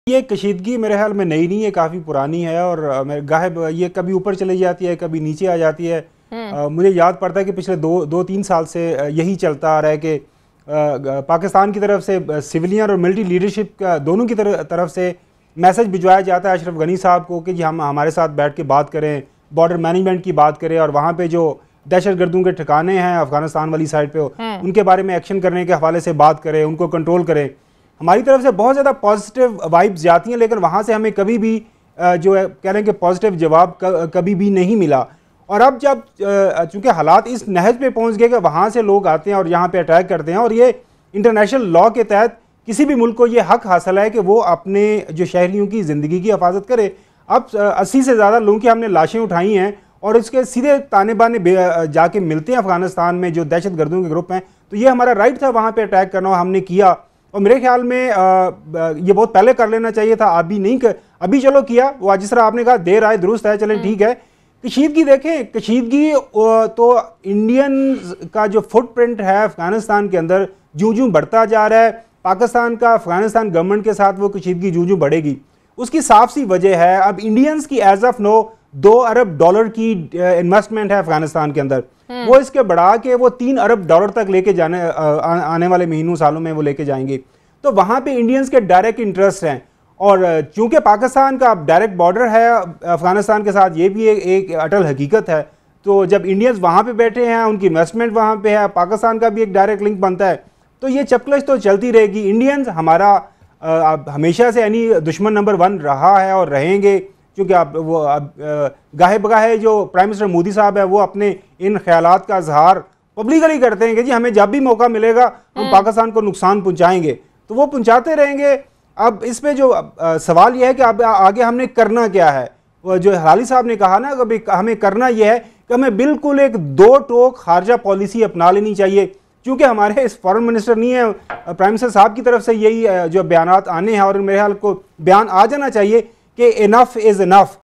India's close ties with Afghanistan will weaken Pak-Afghan relations 19-02-2017 - 92NewsHDPlus
Watch India's close ties with Afghanistan will weaken Pak-Afghan relations 92NewsHDPlus